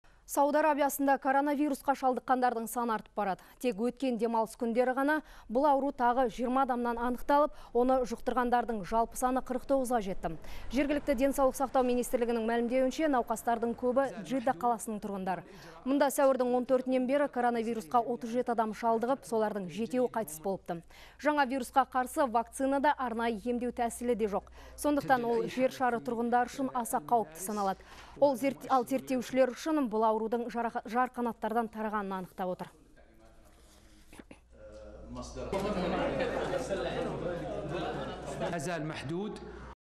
Сауд Арабиясында коронавирусқа шалдыққандардың саны 49-ға жетті
Науқастардың дені - Джидда қаласының тұрғындары. .